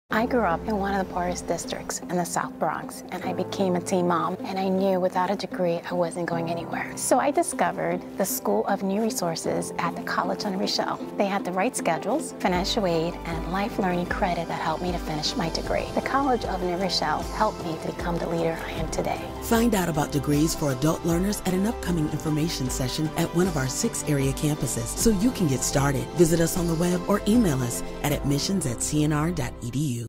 School of New Resources Alum Commercial
Hear how the School of New Resources changed Johanna DeJesus-Cortes' life. The School of New Resources offers 7 convenient New York City and Westchester locations, making it easy to complete your bachelor's degree.